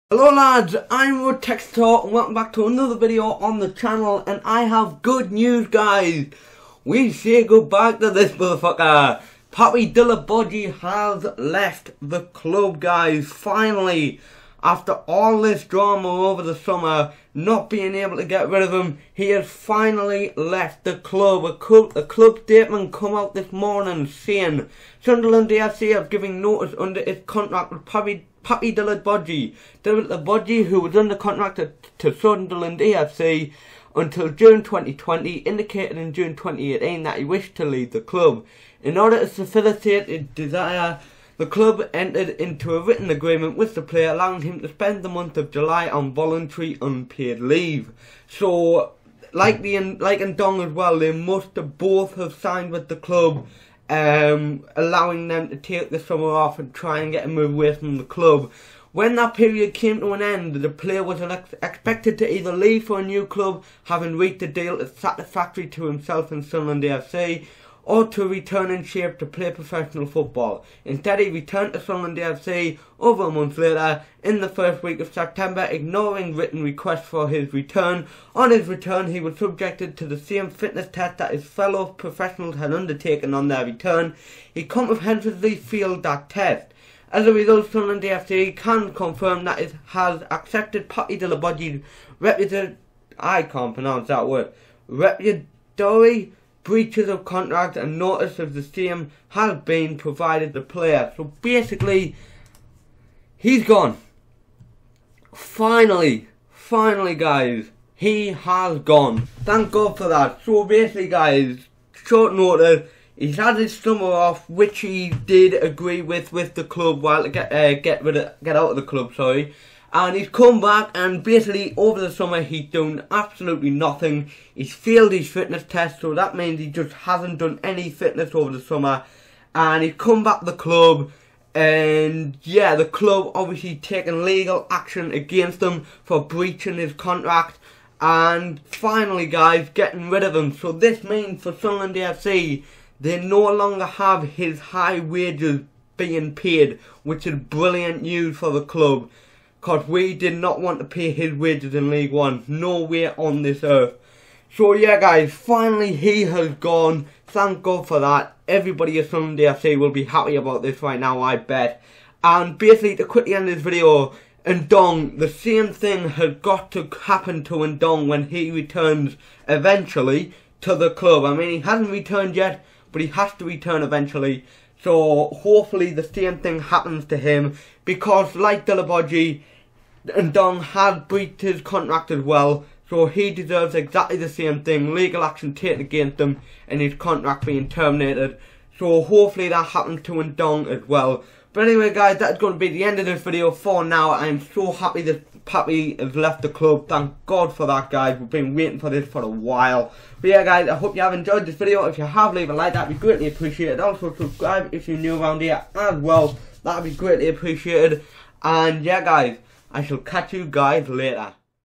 HE'S FINALLY GONE!! | Papy Djilobodji
Finally Papy has left! Hopefully the same thing happens to Ndong!